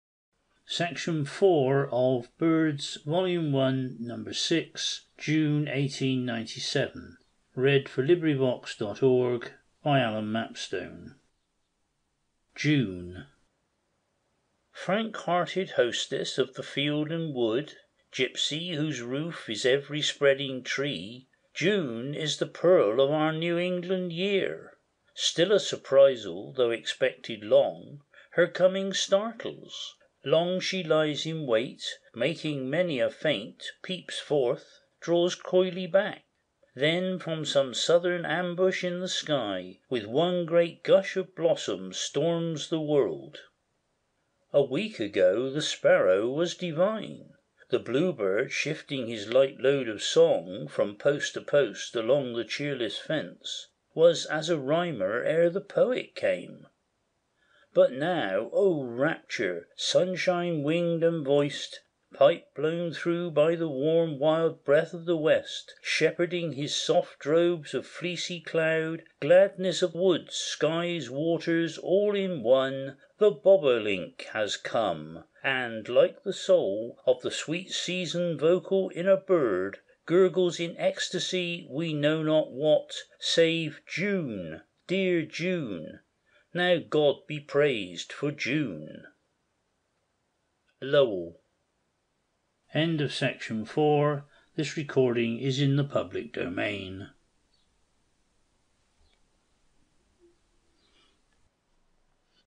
Birds - June 1897: June
BIRDS. (Volume One, Number Six: June, 1897)
Was a monthly publication of the Nature Study Publishing Company of Chicago. It includes short poems and articles describing birds, animals and other natural subjects with accompanying color plates. The magazine was published from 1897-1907 under the various titles, "Birds," "Birds and all Nature," "Nature and Art" and "Birds and Nature." These short pieces make good listening for anyone with a love of nature.